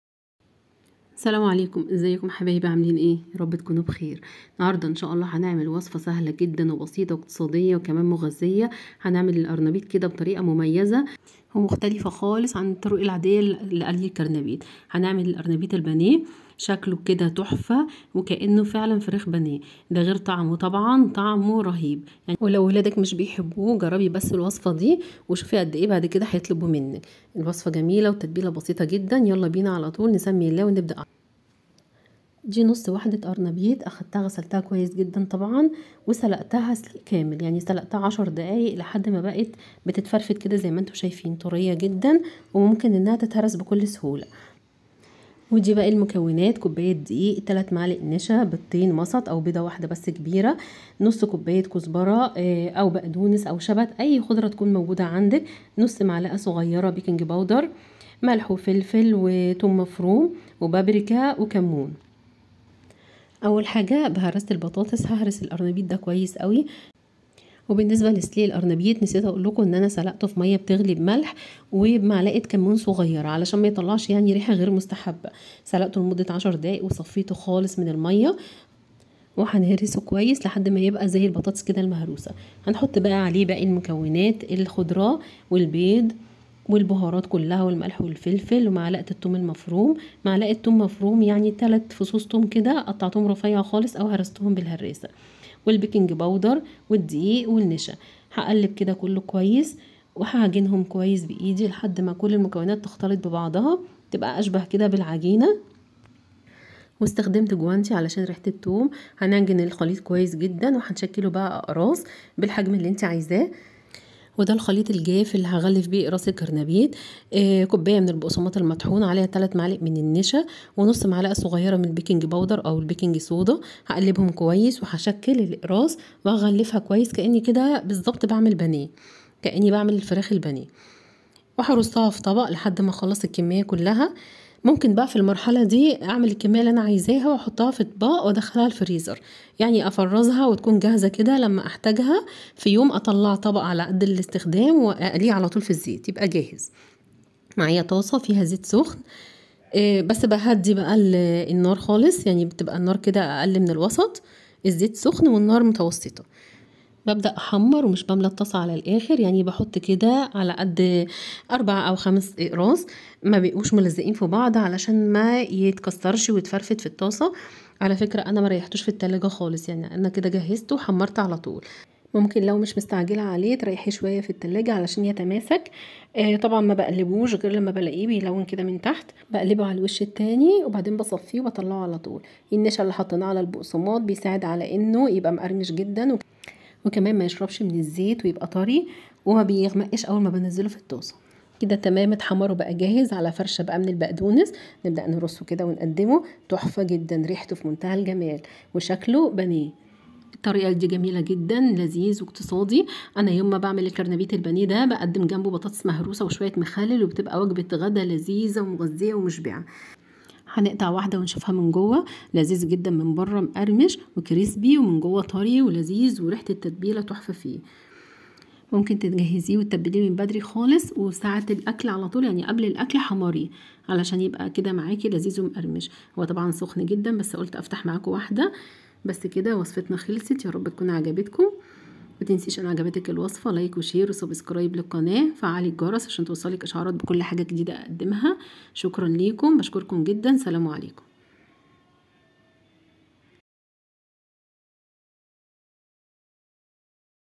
لو مجربتيش الوصفه دى يبقى فايتك كتييييييييير / البانية المقرمش بدون فراخ / لذييييييذ لا يقااااوم 😋👍
طريقه عمل القرنبيط البانيه
قرنبيط مسلوق سلق كامل لمدة عشر دقائق
كوب دقيق
٣ ملاعق كبيرة نشا
٢ بيضه صغيره او واحدة كبيرة
نصف كوب بقدونس مفروم
ملعقة ثوم مفروم
ملح وفلفل وكمون وبابريكا
ملعقة صغيرة بيكنج بودر
زيت للتحمير
التحمير فى زيت ساخن على نار وسط